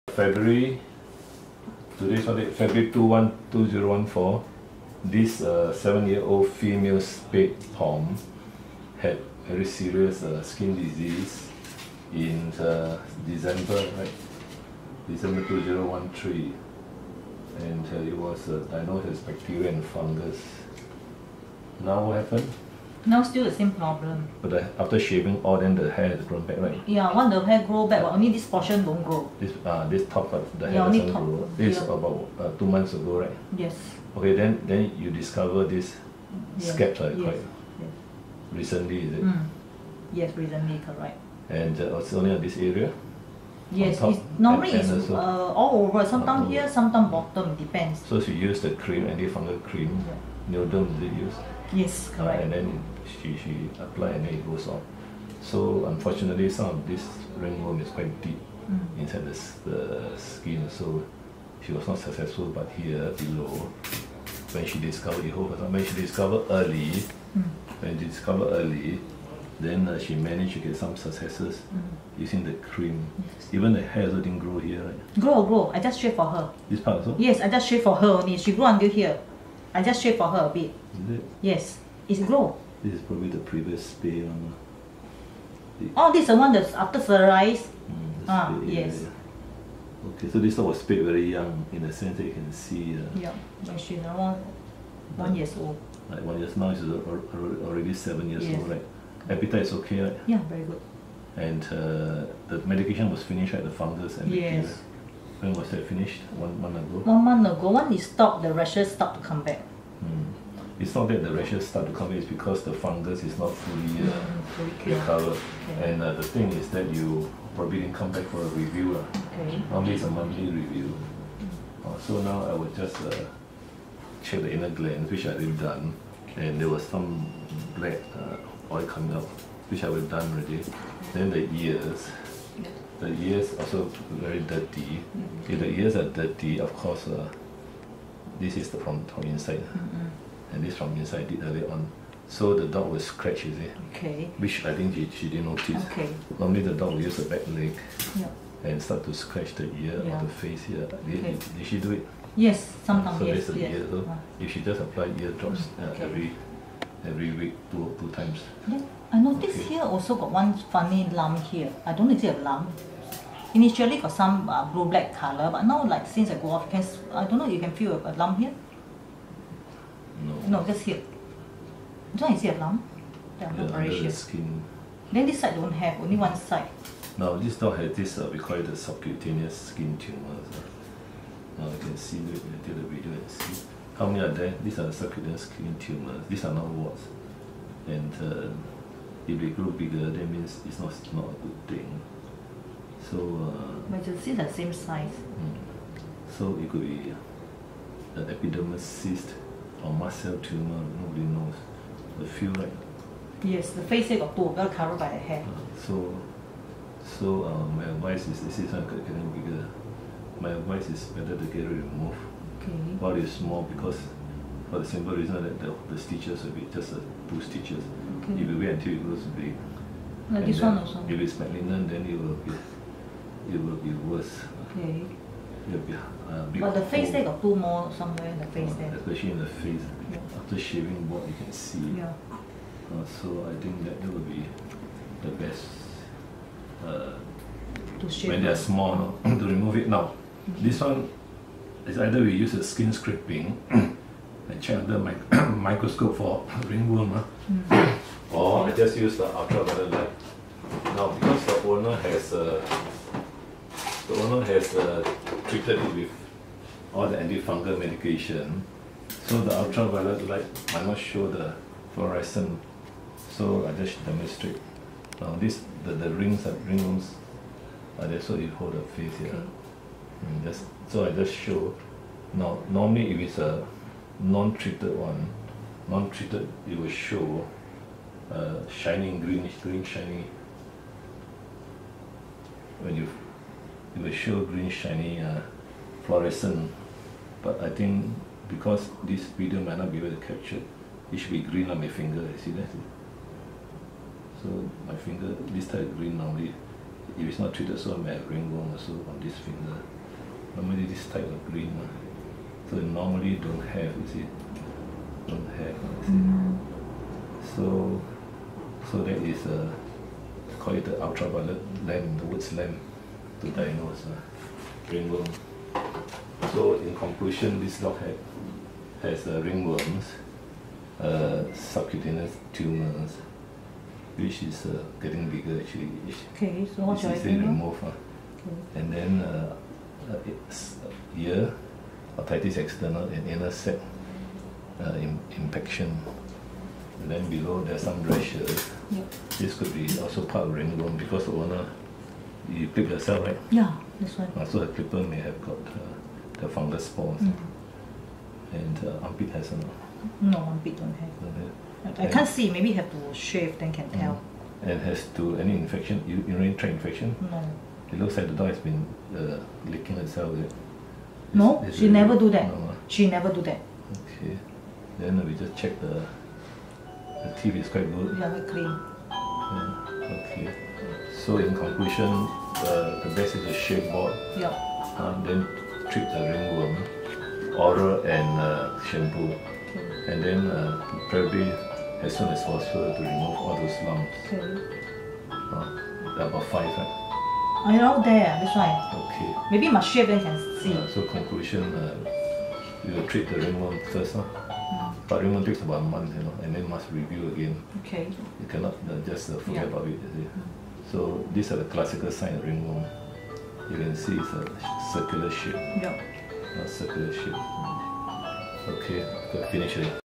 The old Pom has ringworm & 4 skin lumps - 1/2
Ringworm is a fungal skin disease that needs to be reviewed monthly with your vet as it is difficult to get a complete cure in many dogs. After clipping bald 2 months ago for ringworm, the dog has got back his full coat. Except for the bald area on the spinal areas and below them. Ringworm is still present.

The owner knows about ringworm and the dog was clipped bald again.